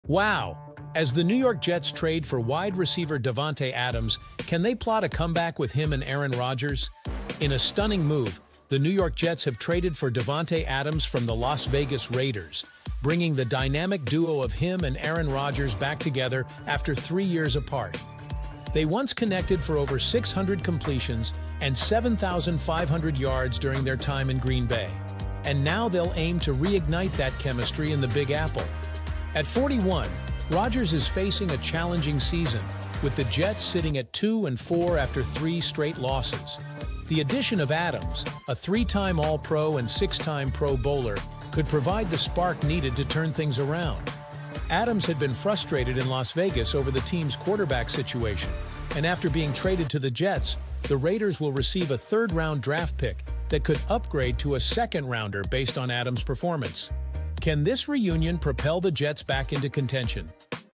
Davante Adams Rejoins Aaron Rodgers as Jets Look to Turn Season Around
🔥 Big moves in New York! Davante Adams reunites with Aaron Rodgers as he’s traded to the Jets! Will this powerhouse duo spark a turnaround for New York? 💥